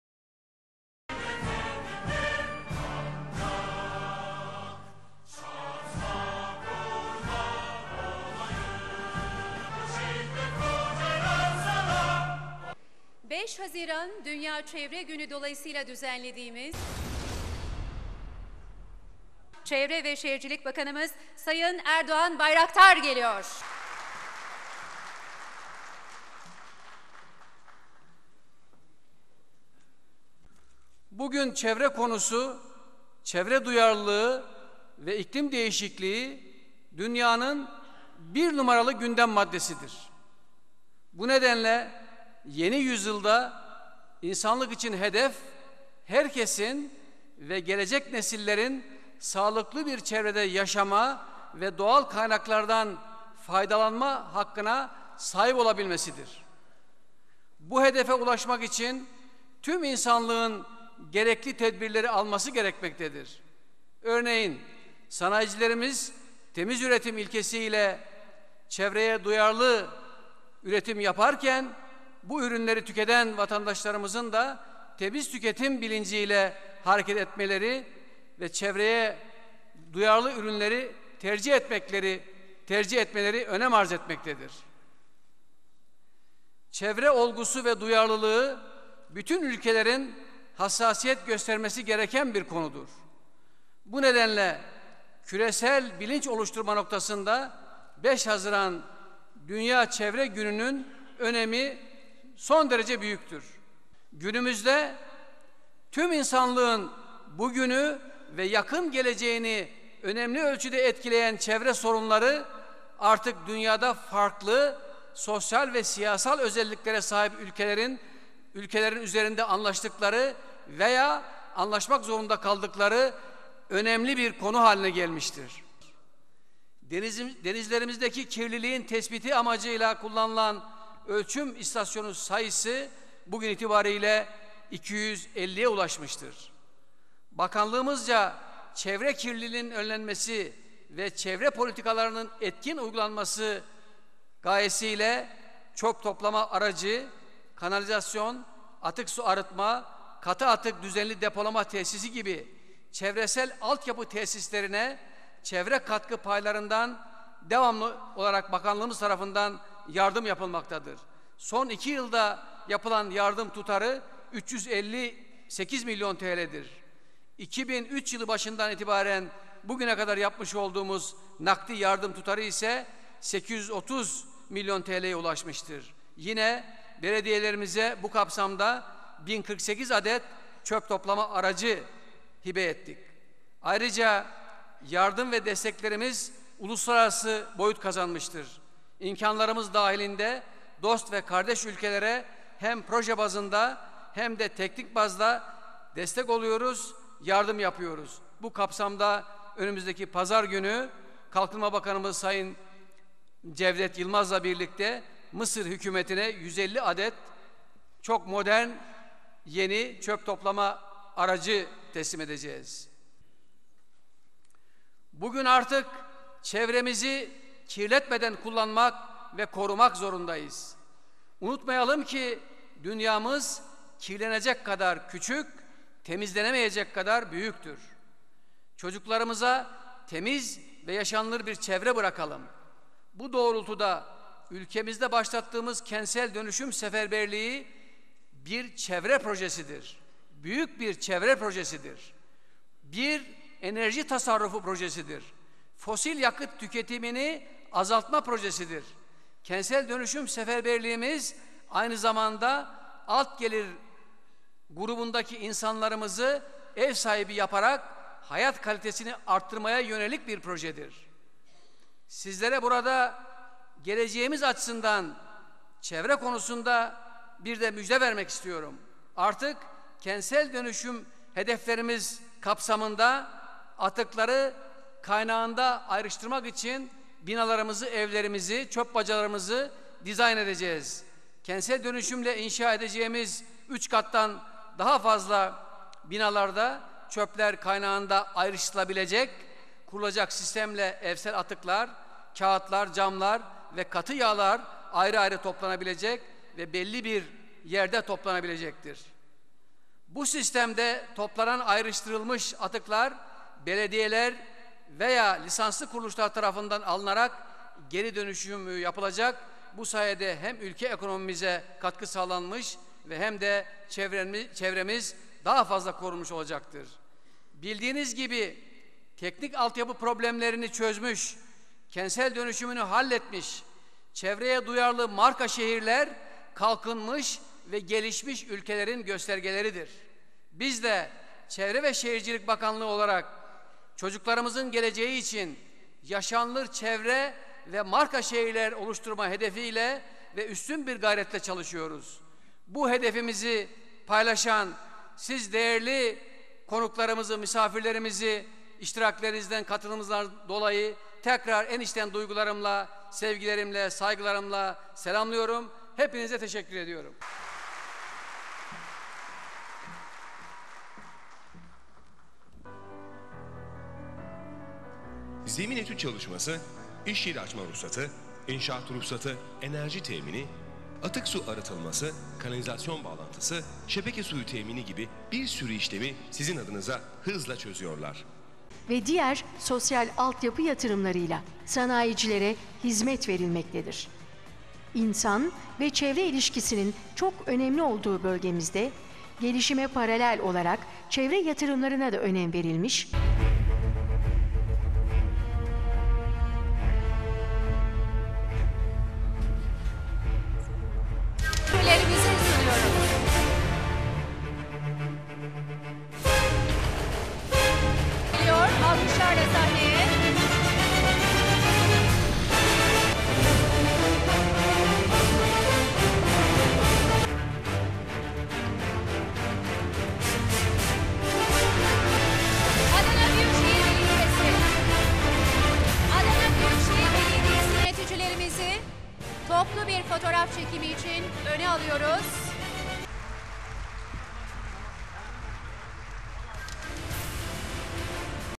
Dünya Çevre Günü Etkinliği
Çevre ve Şehircilik Bakanı Erdoğan Bayraktar, Dünya Çevre Günü etkinliğine katıldı. Etkinlik kapsamında en temiz belediyeler, organize sanayi bölgeleri ve şirketlere ödül verildi. Etkinlikte konuşan Bakan Bayraktar, son yıllarda Türkiye'nin sahip olduğu sosyo-ekonomik dinamizm ve kalkınma ivmesine paralel sürdürülebilir kalkınmanın en önemli bileşenlerinden çevre konulu mevzuat ve uygulamalardaki gelişmelerin takdire değer olduğunu söyledi. 2002 yılında 145 atıksu arıtma tesisi ile 248 belediyeye hizmet verilirken bu rakamın bugün 428 atıksu arıtma tesisi ile 550 belediyeye çıkarıldığını anlatan Bayraktar, "2002 yılında belediye nüfusunun yüzde 35'inin Atıksuyu arıtılmakta iken bugün itibarıyla belediye nüfusunun yüzde 72'sinin atıksuyu arıtılmakta. 2023 yılı itibarıyla bu oran yüzde 100'e çıkacak. 2002 yılında böyle bir uygulama yokken, bugün ülkemizde 10 bin m3 gün üzerinde atıksu deşarjı yapan tüm işletmeler online oılarak izlenmekte." dedi.

"TEHLİKELİ ATI GERİ KAZANIM TESİSLERİNDEN 1 MİLYAR TL KATMA DEĞER SAĞLANDI "

Atıksu arıtma tesislerinin Elektrik giderlerinin yüzde 50'si Çevre ve Şehircilik Bakanlığı tarafından karşılandığına temas eden Bayraktar, "Patlayan çöp yığınlarından katı atık düzenli depolama sahalarına geçilmiştir. 2002 yılında 15 Düzenli Depolama Tesisi bulunmakta iken; bugün itibarıyla 69 düzenli depolama tesisi ile 903 belediyede 44,5 milyon kişiye hizmet verilmekte. Ambalaj atıkları lisanslı toplama-ayırma ve geri dönüşüm tesisleriyle kaynağında ayrı toplanmaktadır. Bu tesislerin sayısı 2003 yılında 28 iken, bugün bu sayı 715'e ulaştı. Tehlikeli atıklar geri kazanılarak ekonomiye kazanç sağlanmakta. 2003 yılında Tehlikeli atık geri kazanım tesisi sayısı 18 iken, bugün bu sayı 201 oldu. Bugün itibari ile Bakanlığımızca lisanslandırılan 1086 geri kazanım tesisinde 60 bin kişi istihdam edilmiş ve geri kazanım faaliyetleri sonucu sağlanan katma değer yıllık 1 milyar TL'yi aşmıştır." şeklinde konuştu.

MAVİ BAYRAKLI PLAJ VE MARİNA SAYISI 374'E ULAŞTI

Denizlerde acil durumlara karşı artık daha hazırlıklı olunduğunu ifade eden Bayraktan şunları söyledi;

"2002 yılında böyle bir uygulama yokken, bugün itibarıyla 1 adet ulusal ve 6 adet bölgesel acil müdahale planımız hazırlanarak bakanlığımız tarafından onaylanmıştır. Ayrıca 219 adet Kıyı tesisimiz acil müdahale planı hazırlanmıştır. Gemi atıklarına yönelik olarak tüm limanlarda atık kabul tesislerimiz inşa edilmiştir. 2002 yılında böyle bir uygulama yokken, bugün itibari ile bakanlığımız tarafından lisanslandırılan 232 Liman atık kabul tesisinde gemi atıklarının alınması hizmeti verilmektedir.Mavi bayrak İspanya, Yunanistan ve Fransa'nın ardından dünya dördüncüsüyüz.Temiz denizlerimizin bir göstergesi olarak, mavi bayraklı plaj ve marina sayısı 2003 yılında 271 iken,Bugün itibariyle bu sayı 374'e ulaşmıştır. Koy ve körfezlerimiz artık atık ve pis sularla kirlenmeyecek. Bakanlığımız turistik amaçlı yat ve teknelerden kaynaklanan atıksuların takibi ve denetlenmesinin sağlanması amacıyla Muğla ve Antalya illerinde elektronik mavi kart uygulamasını başlatmış durumdadır. Denizlerimizdeki kirliliğin tespiti amacıyla kullanılan ölçam istasyonu sayısı 2002 yılında 26 iken, bugün bu sayı 249'a çıkarılmıştır."

Ülke genelinde başlatılan kentsel dönüşüm seferberliğinin, bir çevre, enerji tasarrufu ve fosil yakıt tüketimini azaltma projesi olduğundan söz eden Bayraktar, "Kentsel Dönüşüm Seferberliğimiz, aynı zamanda; alt gelir grubundaki insanlarımızı, ev sahibi yaparak hayat kalitesini arttırmaya yönelik bir projedir. Artık; kentsel dönüşüm hedeflerimiz arasında 'Atıkları Kaynağında Ayrıştırma' var. Kentsel dönüşümle inşa edeceğimiz üç kat ve üzerinde kata sahip binalarımızda; çöpler, kaynağında ayrıştırılacak, kurulacak sistemle; evsel atıklar, kağıtlar, camlar ve atık yağlar ayrı ayrı toplanacak ve belli bir yerde depolanacaktır.Bu sistemde; toplanan ayrıştırılmış atıklar; belediyeler veya lisanslı kuruluşlar tarafından alınarak geri dönüşümü yapılacak; bu sayede hem ülke ekonomimize katkı sağlanmış hem de çevremiz korunmuş olacaktır." dedi.